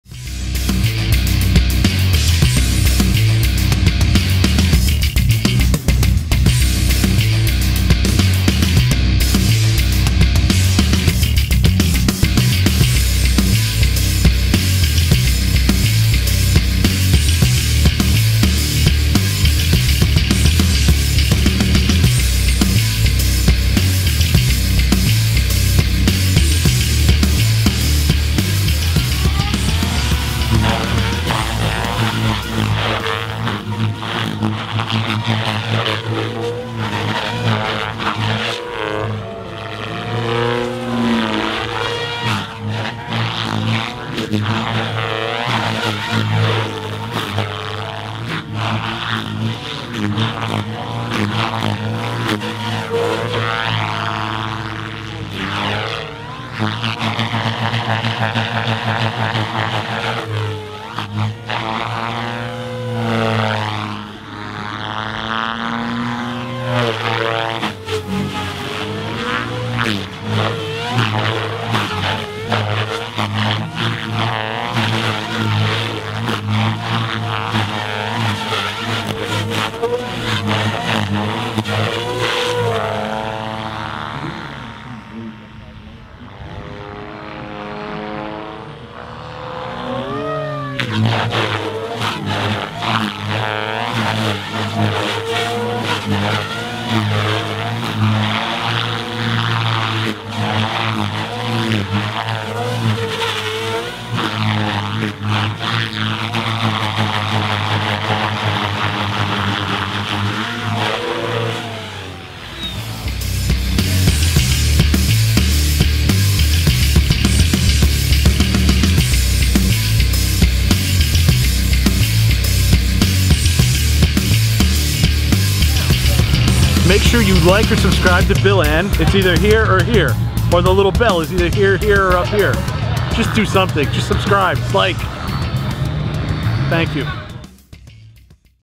Kan Poonnoi flying Protos 380 at IRCHA 2019
Kan Poonnoi putting a fast smack down flight on the Protos 380!
Brought back to life by XLPower, the Protos 380!